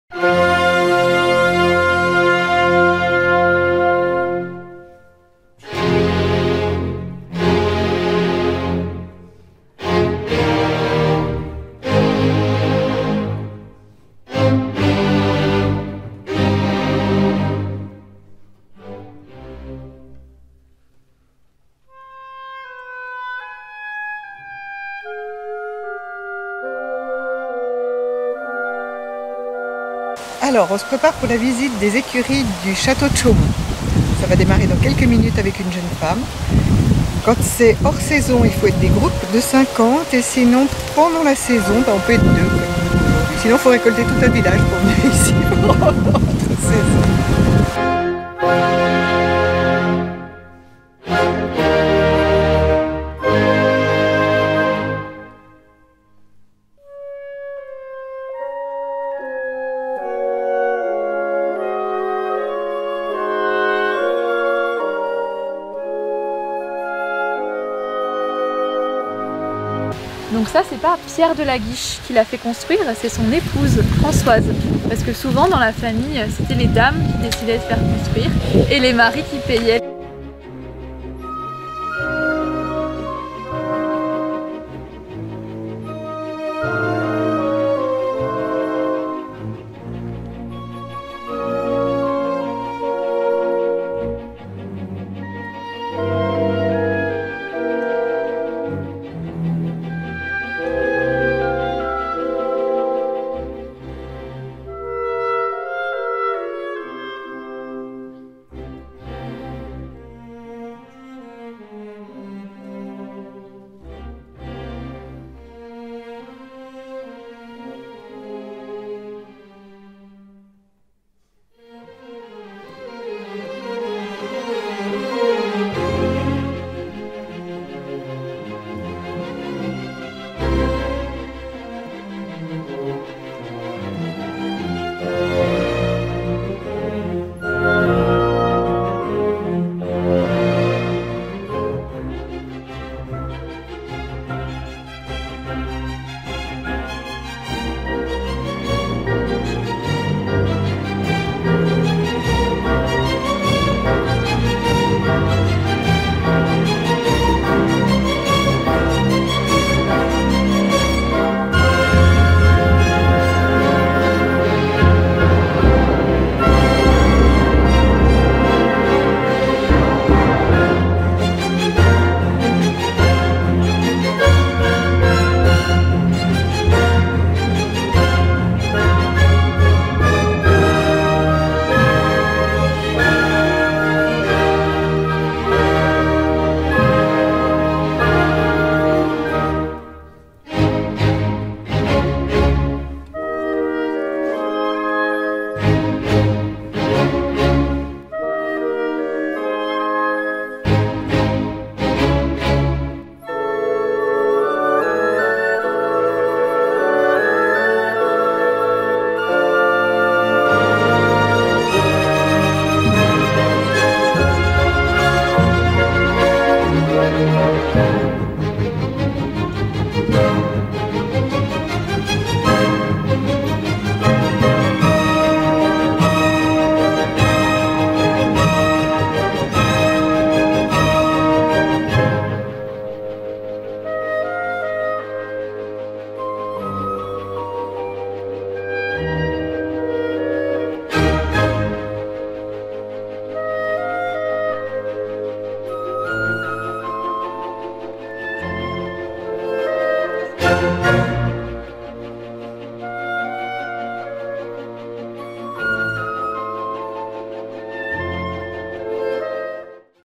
Les écuries de Chaumont-Laguiche
Découvrez le écuries monumentales de Chaumont, construites au XVIIe siècle.